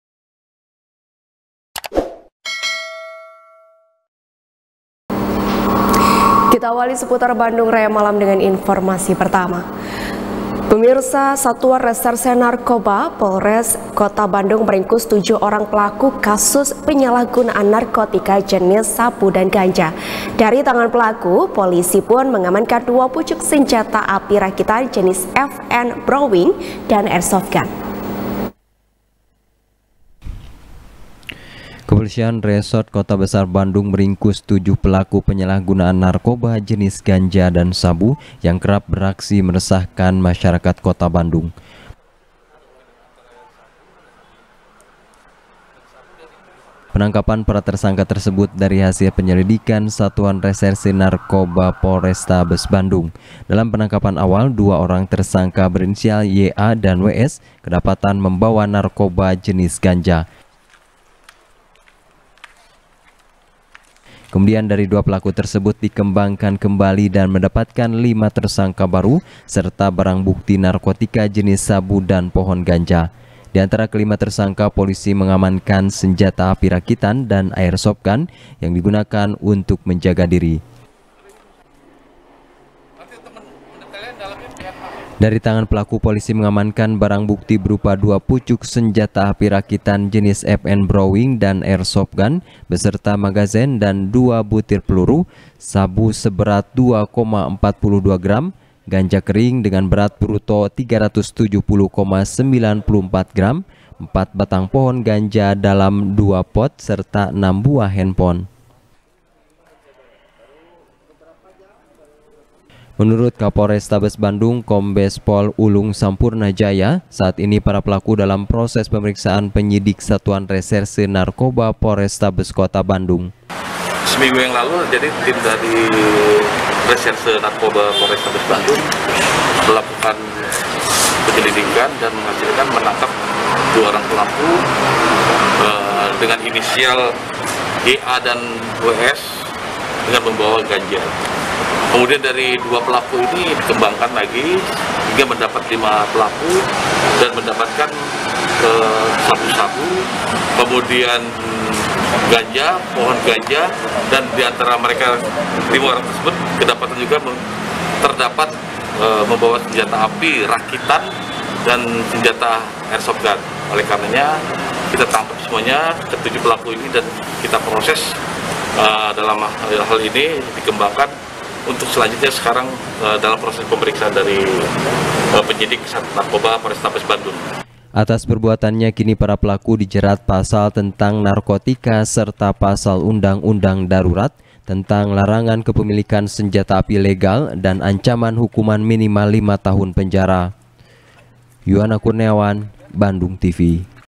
Polrestabes Bandung Ringkus Sindikat Narkoba Bersenjata Api | SBR | BANDUNGTV
SBR | BANDUNGTV | HANY SHANDRA | Satuan Reserse Narkoba, Polrestabes Kota Bandung meringkus tujuh orang pelaku kasus penyalahgunaan narkotika, jenis sabu dan ganja. Dari tangan pelaku, polisi pun mengamankan dua pucuk senjata api rakitan jenis FN browing, dan airsoft gun.